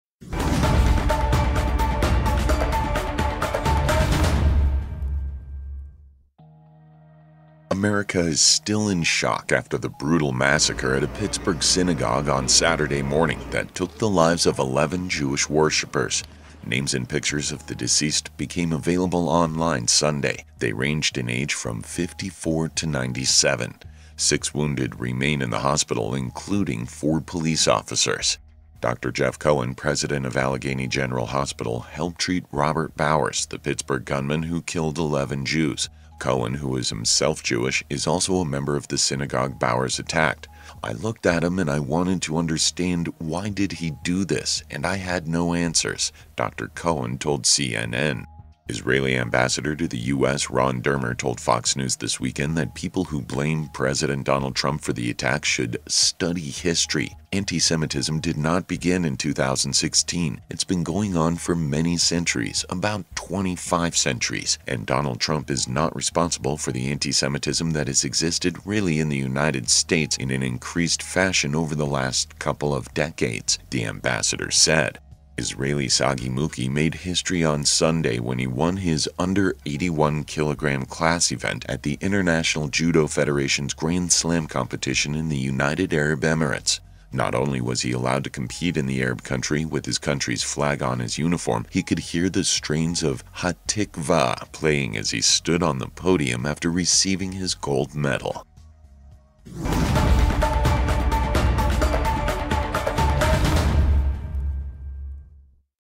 WIN Daily News Recap - Oct 30, 2018
Here is a recap of the top Israel news stories on October 30, 2018.